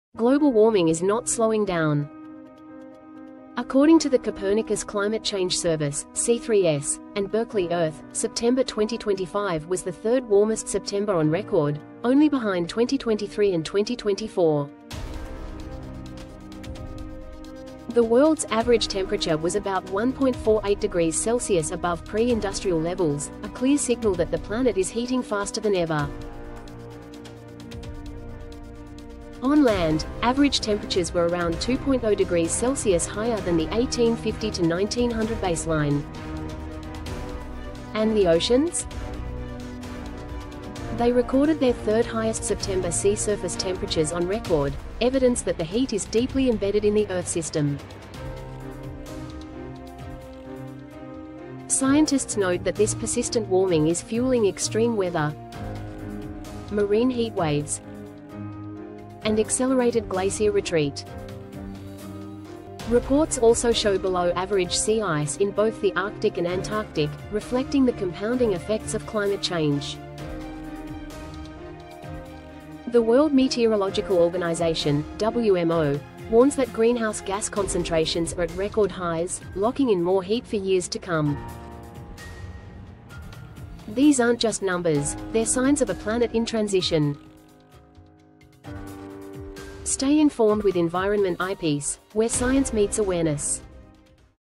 Global Warming Hits New Highs: September 2025 Update
🌡️ September 2025 was the third-warmest September ever recorded.
Global temperatures reached about 1.48 °C above pre-industrial levels, and oceans logged one of their hottest months in history.
Scientists warn this trend drives extreme weather, marine heatwaves, and ice melt worldwide.
Sources: Copernicus C3S (2025), Berkeley Earth (2025), WMO (2025)
Base map: Temperature anomaly data © Berkeley Earth (September 2025). Used for educational reporting purposes.